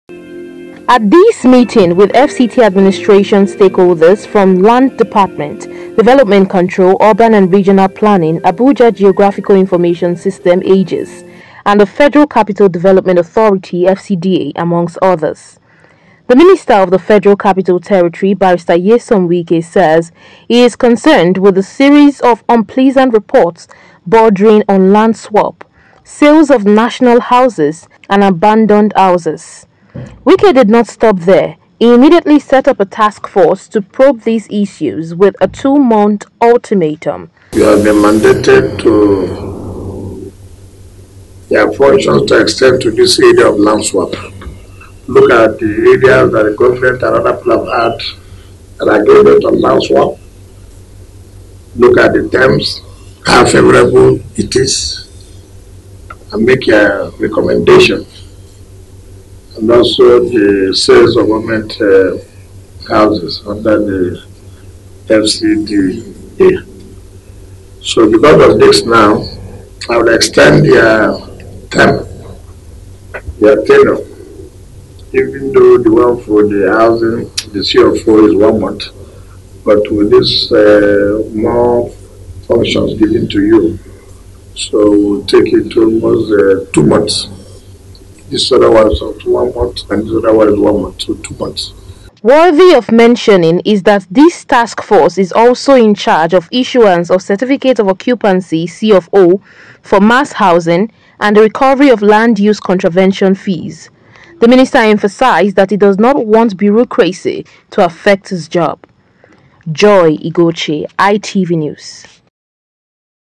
Land Swap: Wike Sets-Up Taskforce, Issues 2-Month Feedback Ultimatum
Amidst Series Of Reports Bordering On Land Swapping And Abandoned Houses In Abuja, The Minister Of The Federal Capital Territory (FCT), Barrister Nyesom Wike, Has Inaugurated A Task Force To Investigate And Report To Him In Two Months.

Wike Stressed That If The Task Force Is Given Luxury Of Time, They Will Give In To Complacency.